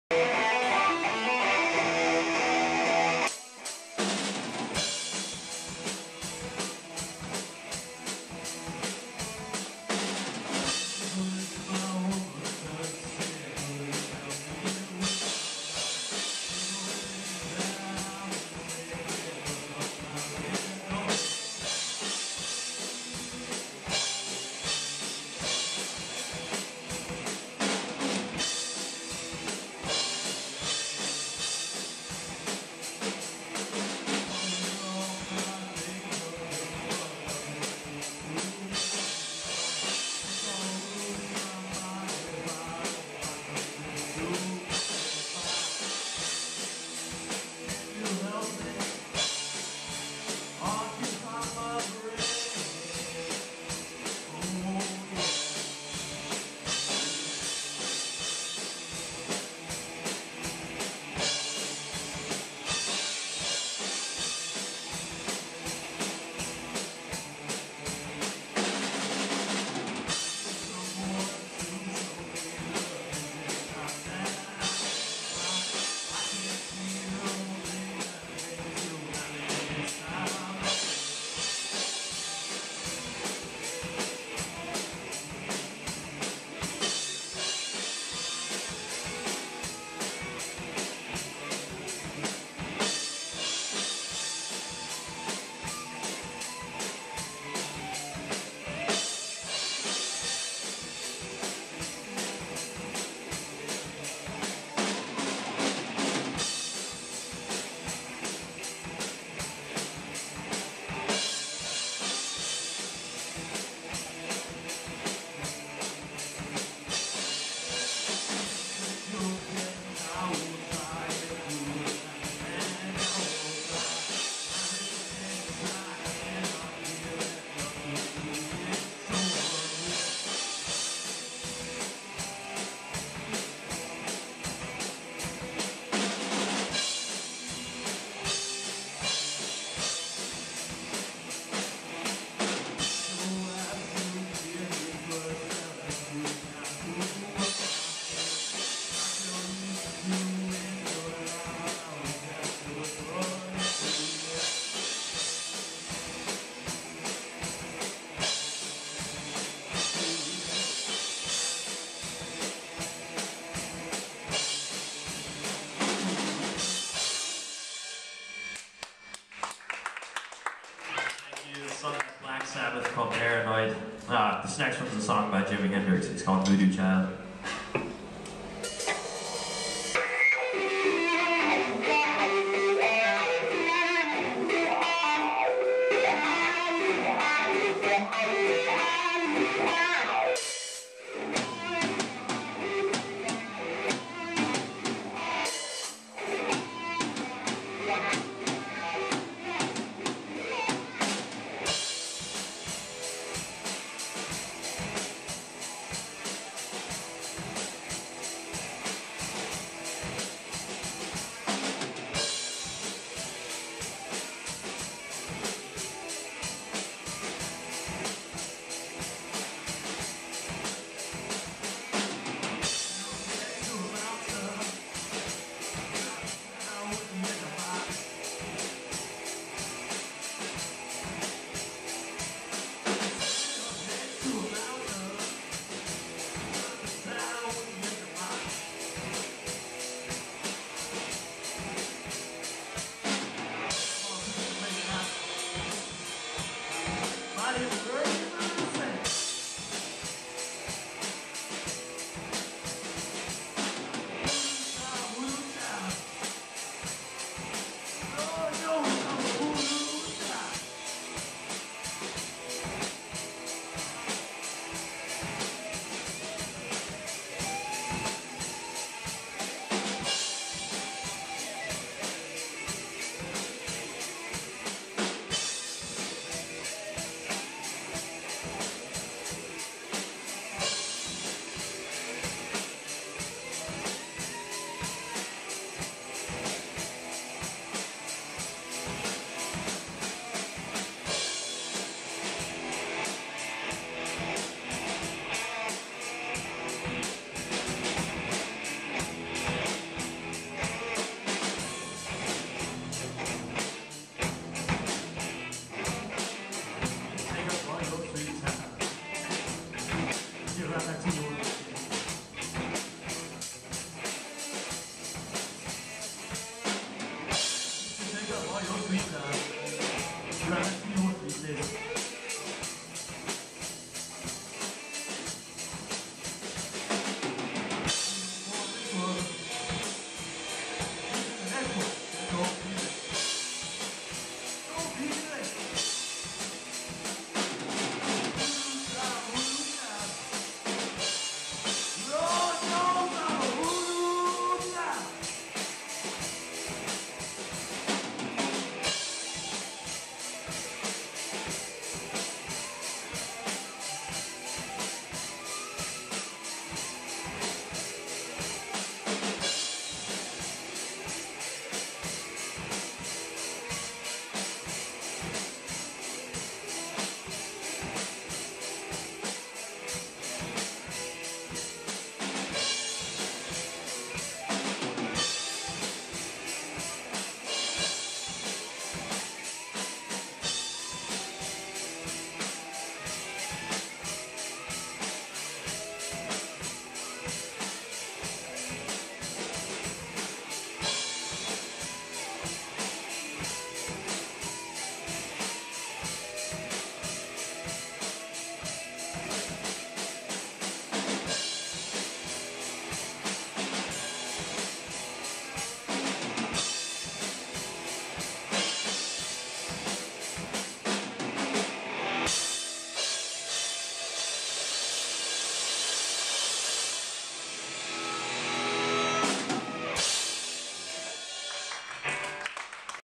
Scinite plays Veracity
Scinite plays the May Veracity show at the Vera Project 5-9-12